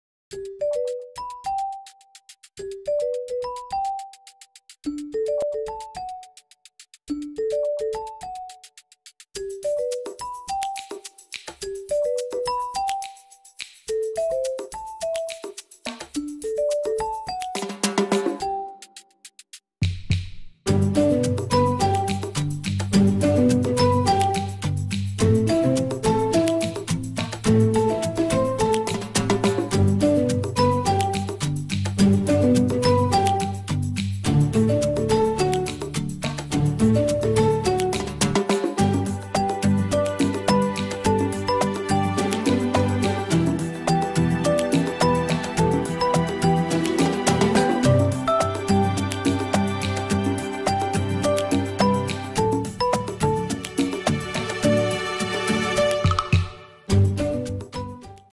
Pizzelle di alici (Sofficissime) - I fornelli di Max
A me calde calde fanno uscire pazzo... e a voi ???

E oggi ? cosa cucino ? 🤷‍♀️🤦‍♂️
Video ricetta SVUOTA FRIGO!
Un idea quando non sappiamo proprio cosa cucinare!!! 🏃‍♀️🏃‍♂️

Ingredienti
250 gr farina
300 gr acqua
50   gr parmigiano
70   gr alici dissalate
2     gr sale
7     gr lievito  istantaneo

𝐂𝐨𝐧𝐝𝐢𝐯𝐢𝐝𝐢 𝐥𝐚 𝐫𝐢𝐜𝐞𝐭𝐭𝐚 𝐬𝐮𝐥𝐥𝐚 𝐭𝐮𝐚 𝐛𝐚𝐜𝐡𝐞𝐜𝐚
Segui le mie ricette anche su